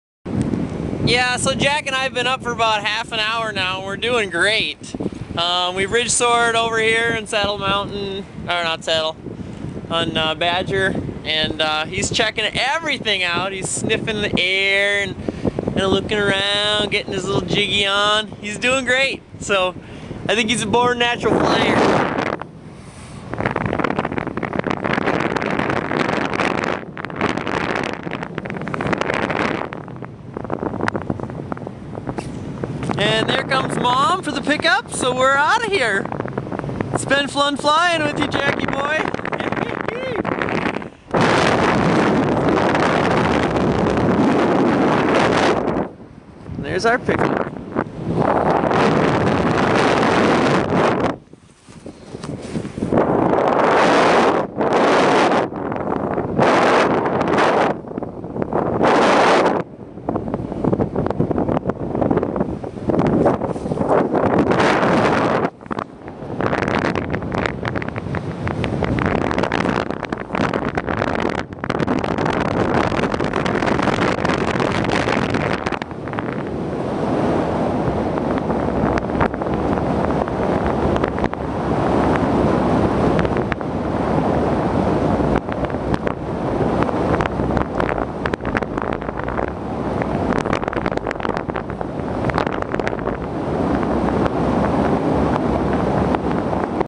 Jack and AP Paraglide Badger Mtn
Jack the flying dog and AP Paraglide Badger Mtn